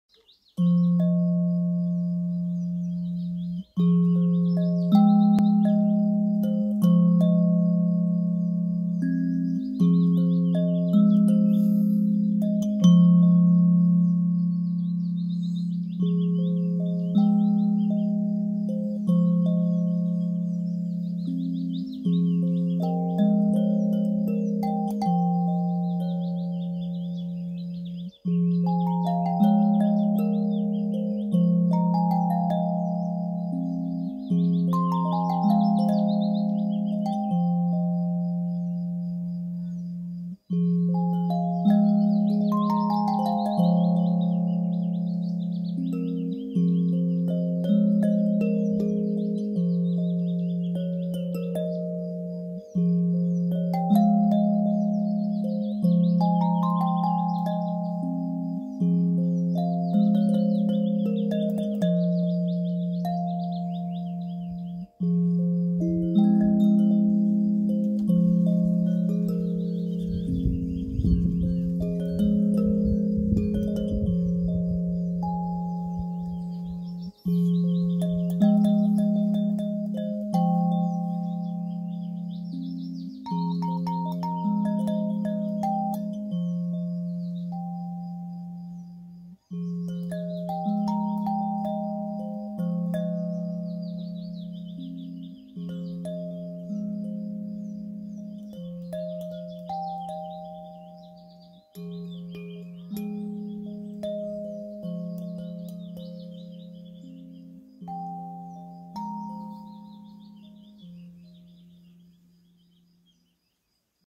19 tone pick up kalimba Olea, diatonic scale F minor, 432 Hz
19 tone electro acoustic kalimba Olea tuned in Fminor,432 Hz.
Walnut wood, cherry veneer on the back side.
This kalimba got only line oil treatment and was hand sanded for 4 hours.
Possible to retune in any diatonic scale from Fmajor/minor to A major/minor.
Long sustain,meditative sound.
Truly Beautiful.